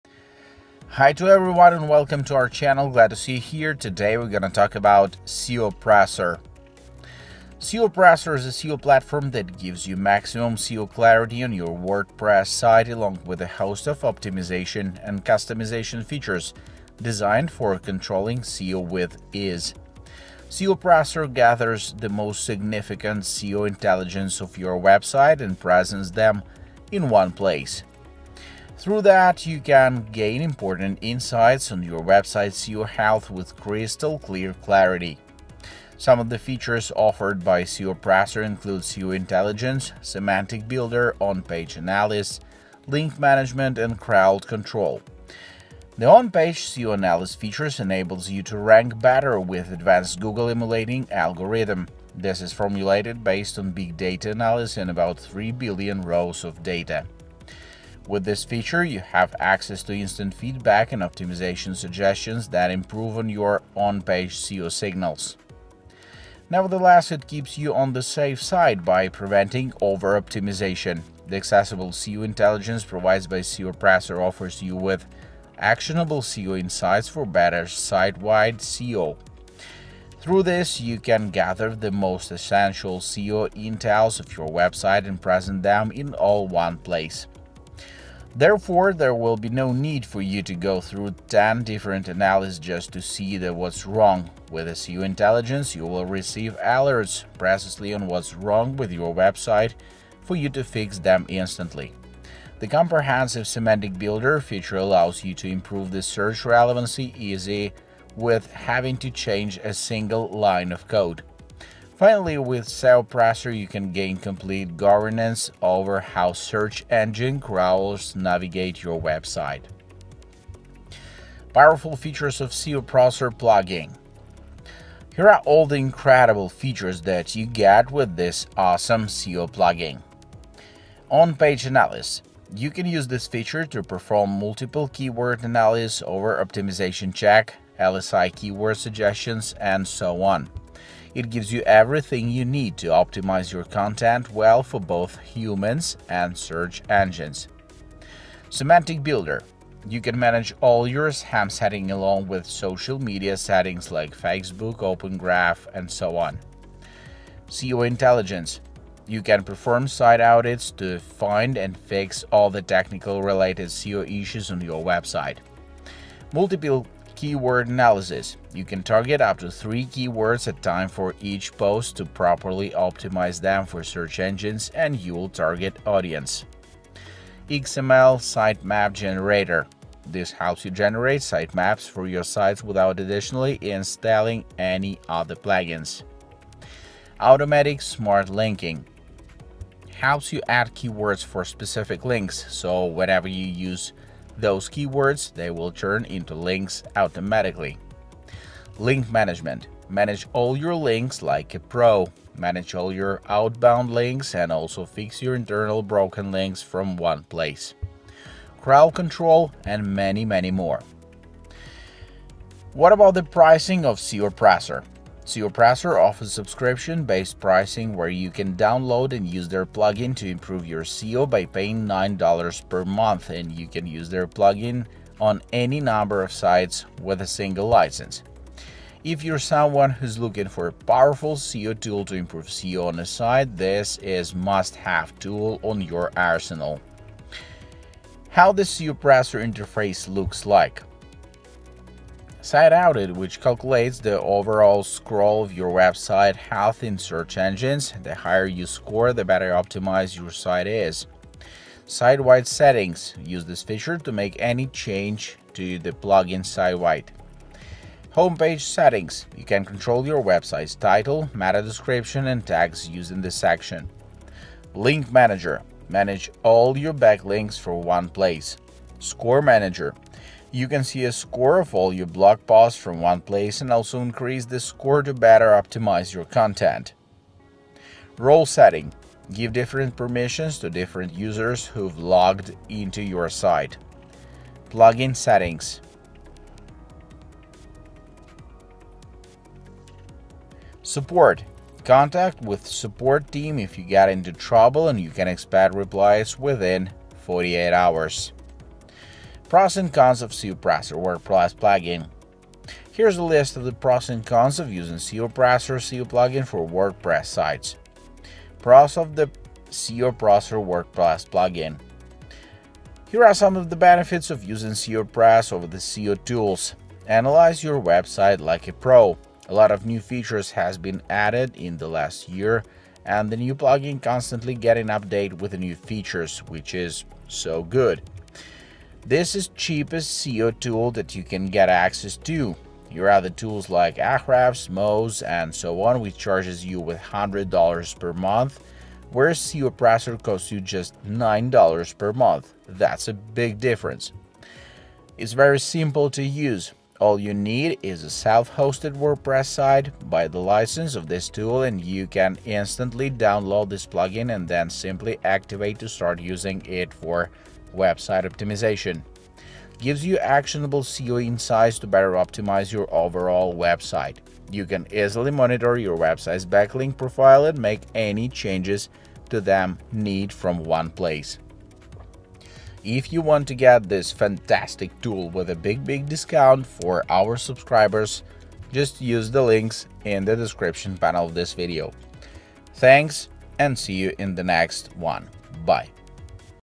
TOP SEO PLATFORMS - THE BEST Wordpress SEO Plugins
Hi Guys!

If you do a quick internet search, you'll find several SEO tools out there, but which is the best SEO tools? This is a bit tricky, especially if you don't know what to look for.

In this video, I cover an in-depth review of top seo tools. If you have ever wondered if SEO tools - Seopressor was worth it or not, by the end of this video, hopefully, you'll be able to decide whether or not it is right for you.

In my opinion, if you're looking for a lead generation solution that is easy to use, cost-effective and performs well, then I think this is a great option for you.

If you're curious to find out the best SEO tools, then take the time to watch this video now!